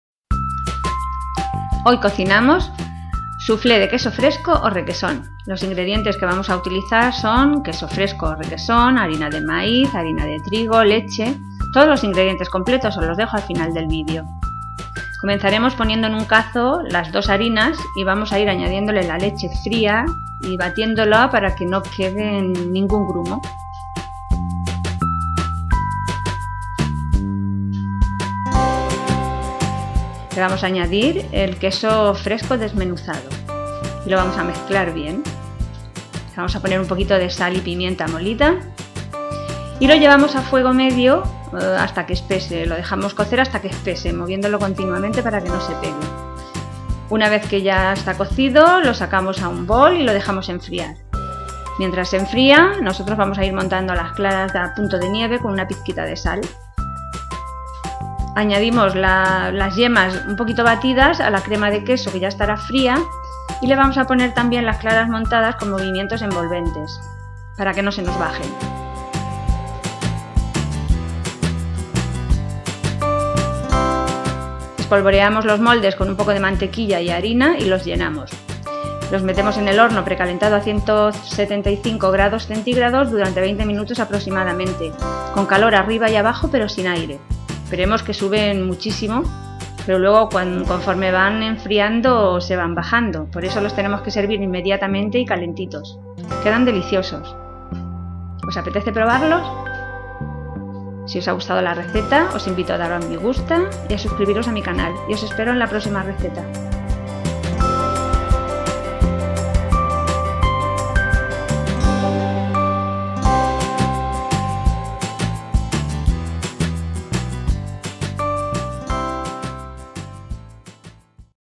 Cómo hacer soufflé de queso fresco o requesón | Receta fácil de soufflé de queso
El soufflé es un plato de origen francés , muy ligero que se realiza con una salsa bechamel como base a la que se le añaden las claras de huevo montadas a punto de nieve.
Se pueden realizan en dulce o en salado, y su característica principal es que mientras lo horneamos el soufflé sube mucho, suele doblar su volumen, pero cuando enfría baja en pocos minutos...
Ingredientes para 4 personas:
     -   125g de queso fresco o requesón
     -   3  huevos M
     -   1/2 vaso de leche (125 ml)
     -   1 cucharada sopera de harina de trigo normal
     -   1 cucharada de harina de maíz refinada (Maizena)
     -   mantequilla y harina para los moldes